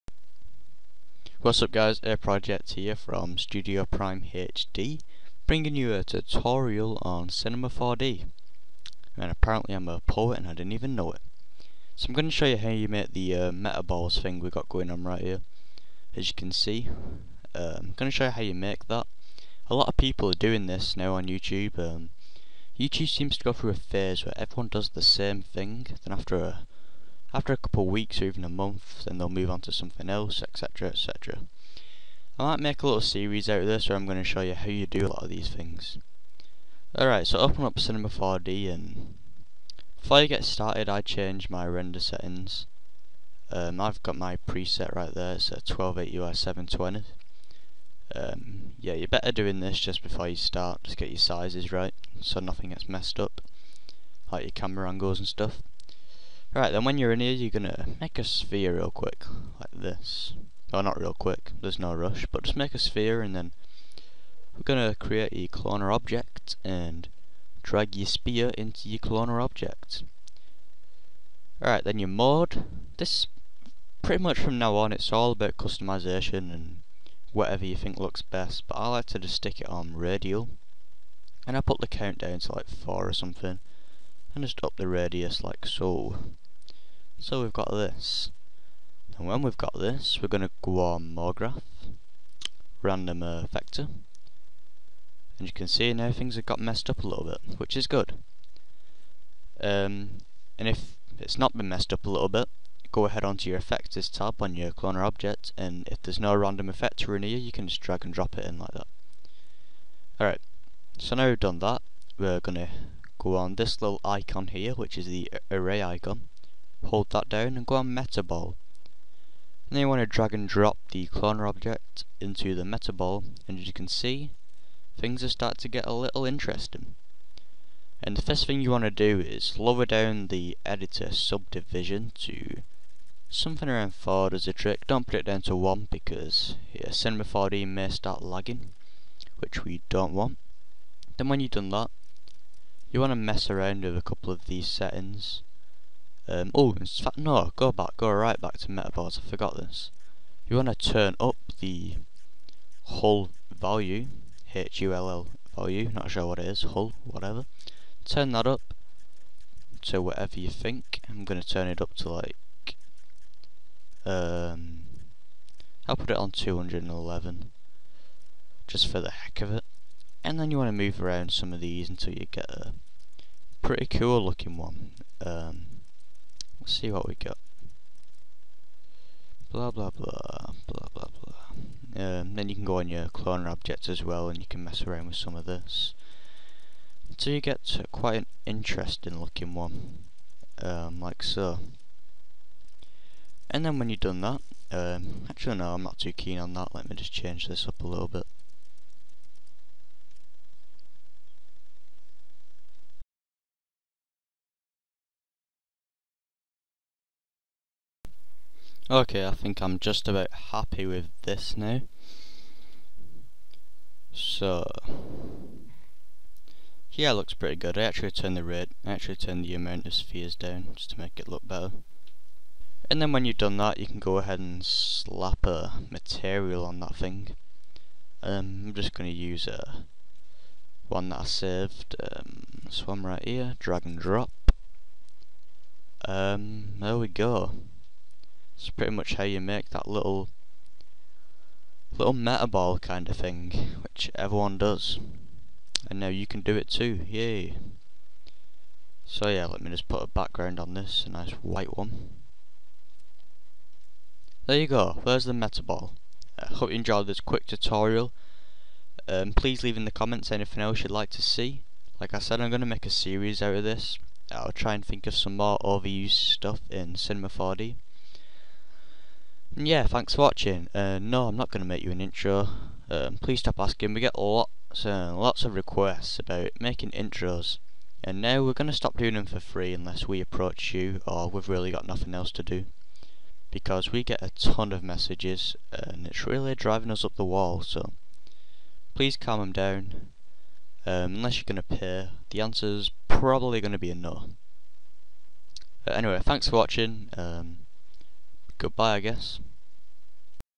"Same Old" Ep1 | Metaball - C4D Tutorial
A new series were I will show you how to do over-used effects/animations used in intros and stuff. And so all the kids dont cry about me 'giving things away' I wont release a tutorial until the effects die down in the community a bit.

And any request will be taken in to consideration.